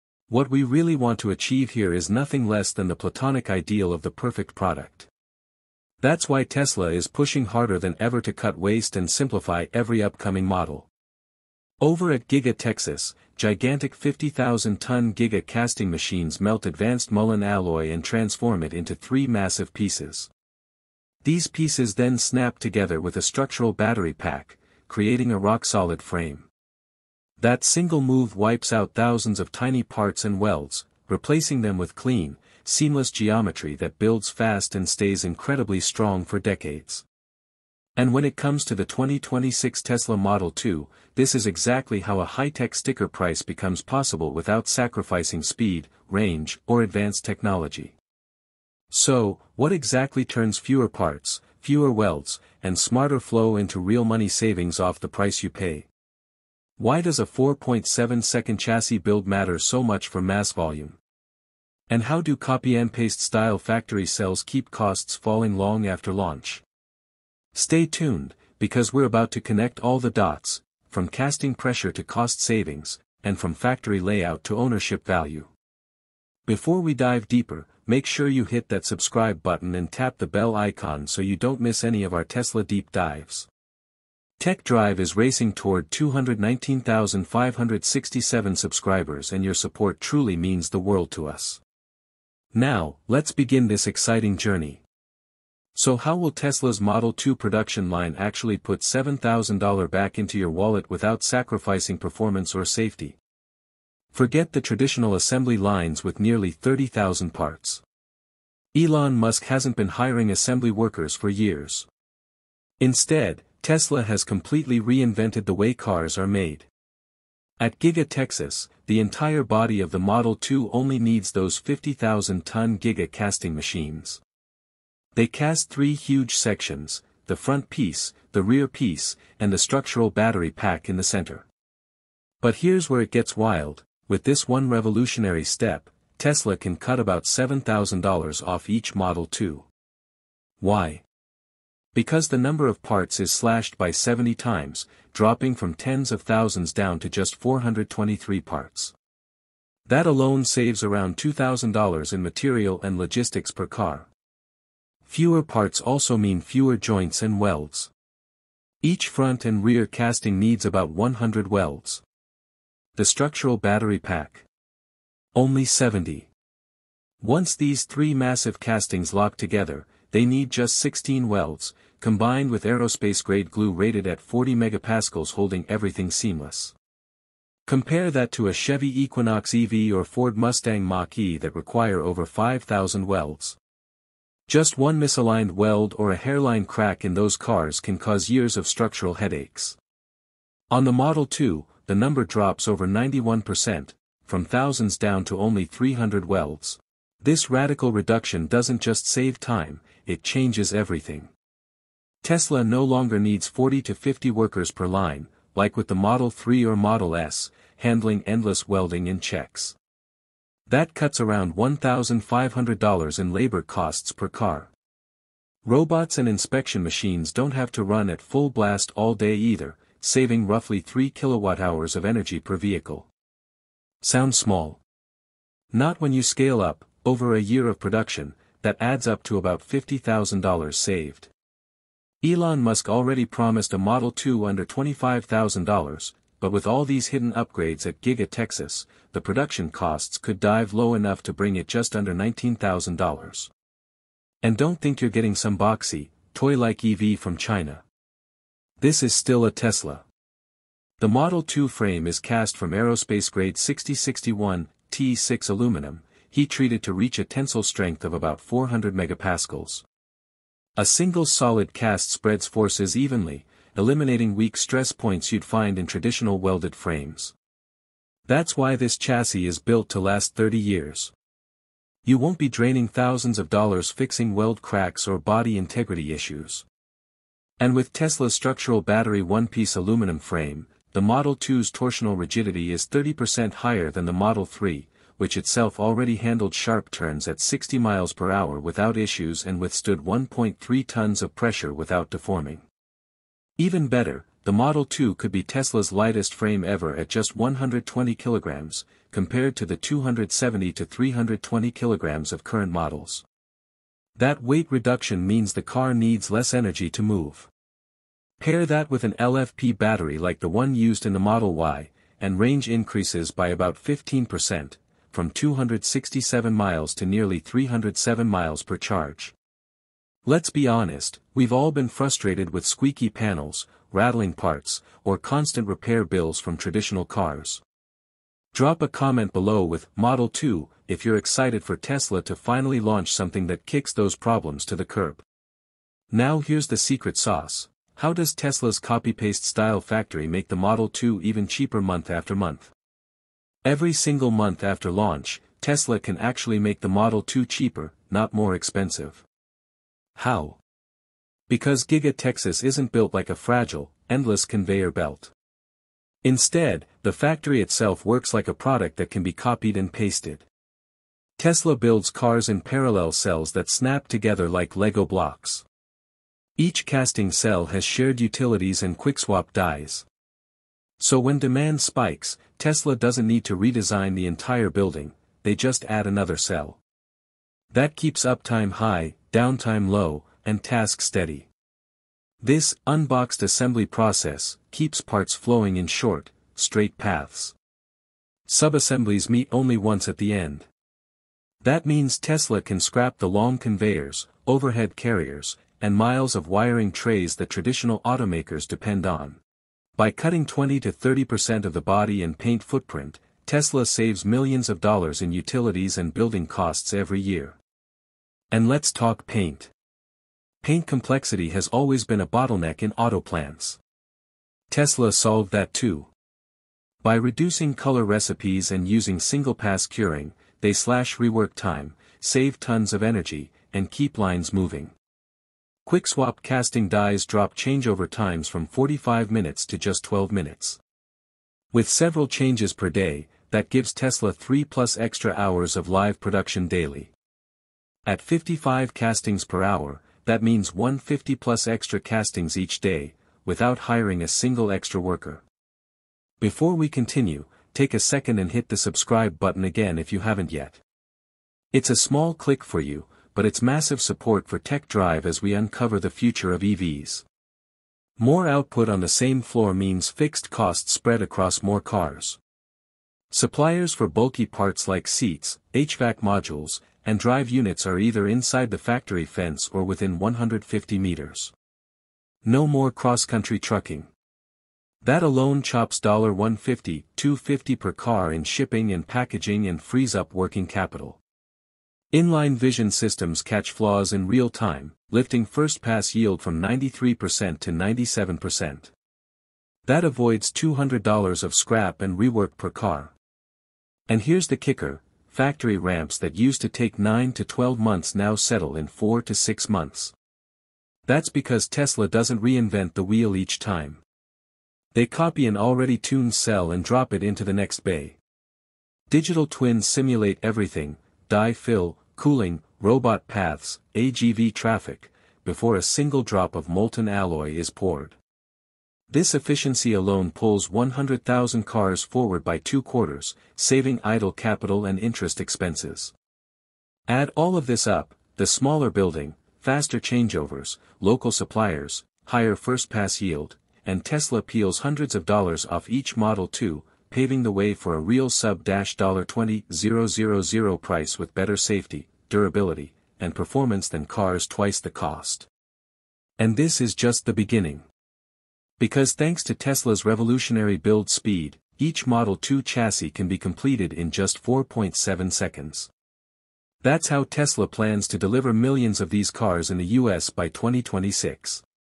🚨 2026 Tesla Model 2: Elon Musk’s $19,000 EV Breakthrough | How Tesla Made It Affordable
The long-awaited 2026 Tesla Model 2 is finally here — and Elon Musk has delivered on his promise of an affordable electric car! With Giga Texas’ 50,000-ton Giga Press, Tesla has revolutionized car production by reducing thousands of parts into just three mega castings. This breakthrough slashes costs, boosts durability, and makes the Model 2 available for under $19,000 — without sacrificing performance, range, or safety.

In this video, we uncover:
✅ How Tesla saves $7,000 per Model 2 with casting innovation
✅ Why the Model 2 can be built in just 4.7 seconds
✅ The factory “copy-paste” system that cuts costs every month
✅ Real range upgrades — over 300 miles per charge
✅ How RoboTaxi + FSD V14 could earn you $35,000/year
✅ Ownership savings from energy, maintenance, and resale value

Tesla isn’t just making an affordable EV — they’re creating a car that pays YOU back.

👉 Drop “Model 2” in the comments if you’re excited for launch!